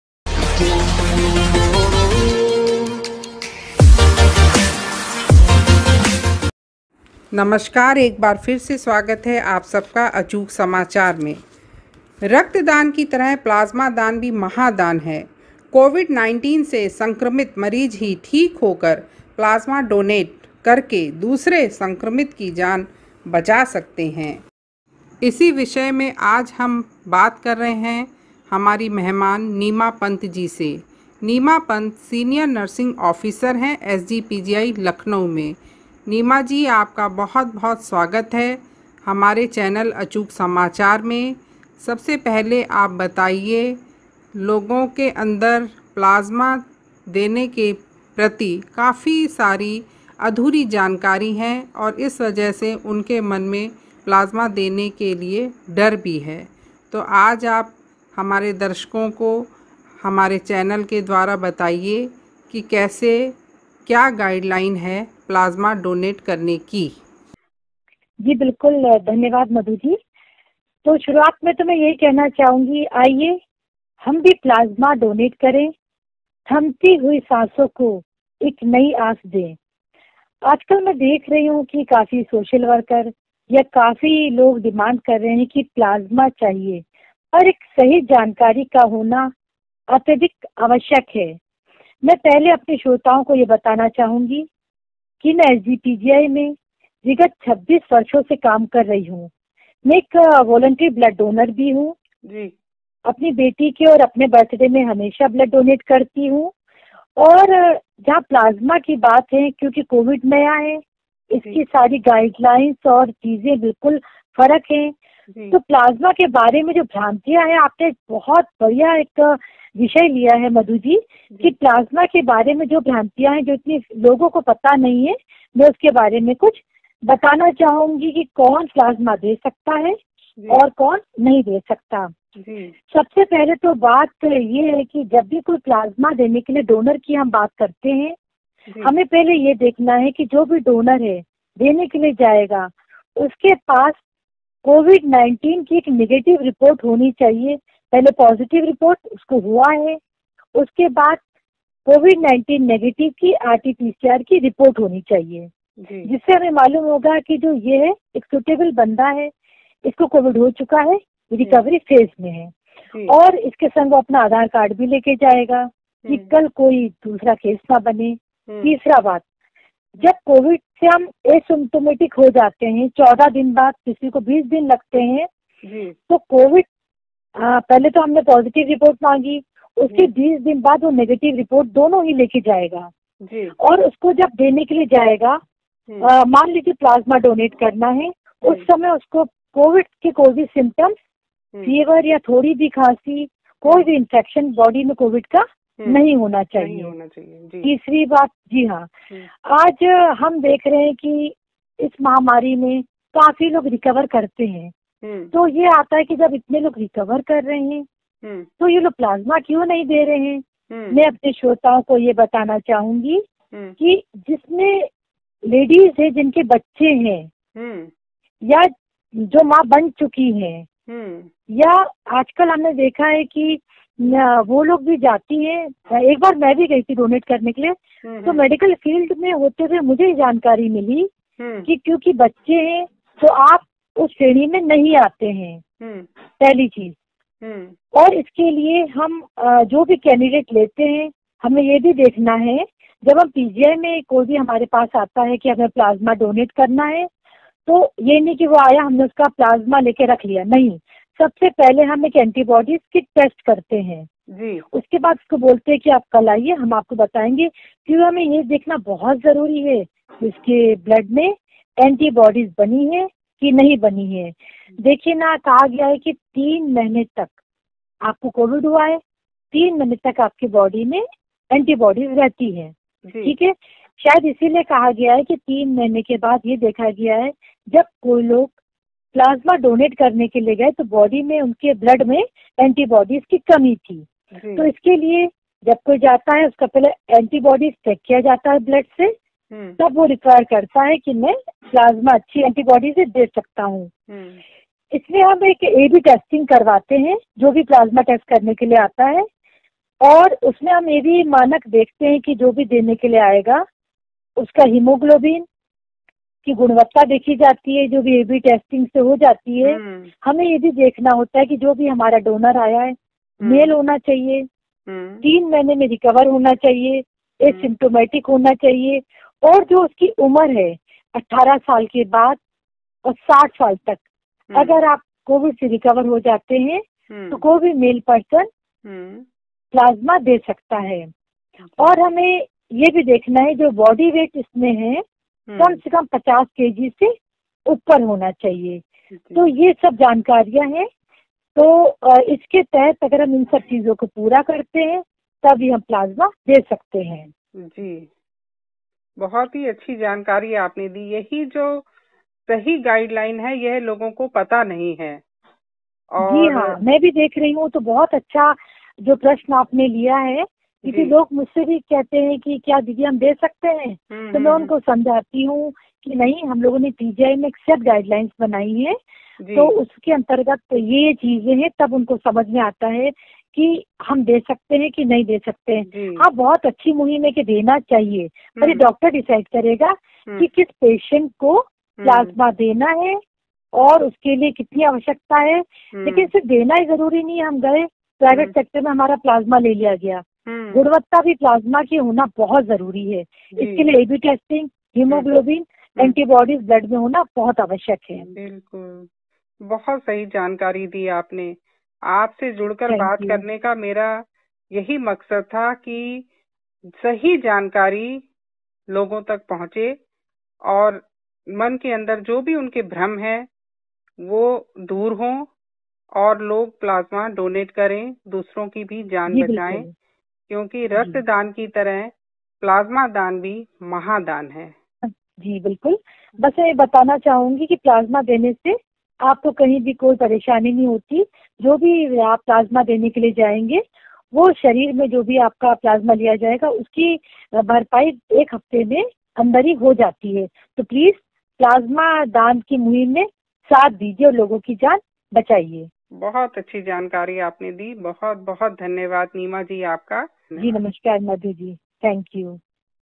रक्तदान महादान के बाद प्लाज्मा(Plasma) दान भी महादान है
Neema Pant SGPGI

प्लाज्मा 18 वर्ष से ऊपर की आयु के पेशेंट डोनेट कर सकते हैं
पेशंट का वजन 50 किलो से ऊपर होना चाहिए
गर्भवती स्त्री या मां बन चुकी स्त्री प्लाज्मा डोनेट नहीं कर सकती
कोई भी कोरोना के लक्षण नहीं होने चाहिए
शरीर में एंटीबॉडीज की कमी होने पर भी प्लाज्मा डोनेट नहीं कर सकते
 रक्तदान महादान के बाद प्लाज्मा दान भी महादान है
प्लाज्मा दान करके आप दूसरे को जीवनदान दे सकते हैं
वैक्सीनेशन कराने के 28 दिन बाद ही प्लाज्मा डोनेट कर सकते हैं